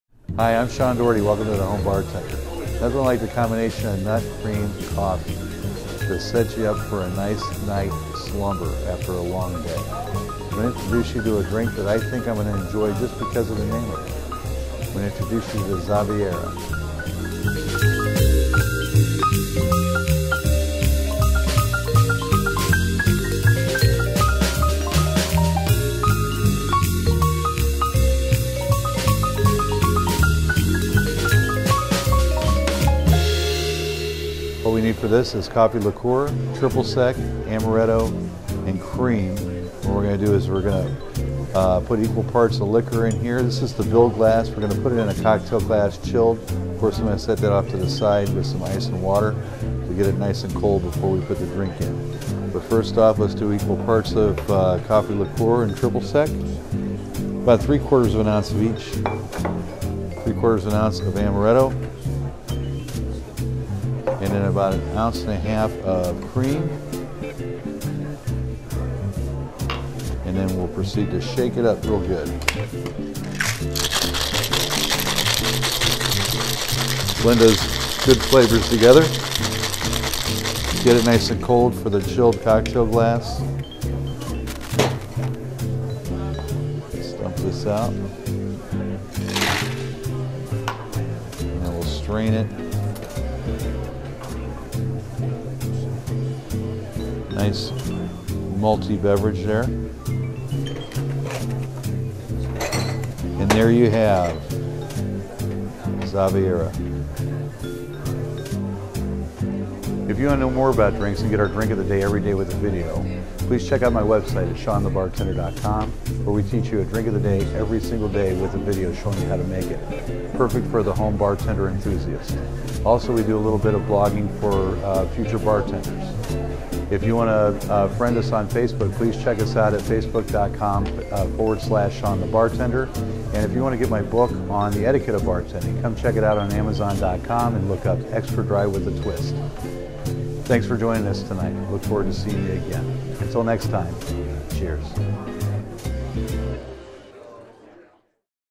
How to make a Xaviera
A creamy beverage that would go perfect with dessert.  Get ready for slumberland after a couple of these.  This will help you take the plunge after a day of running around.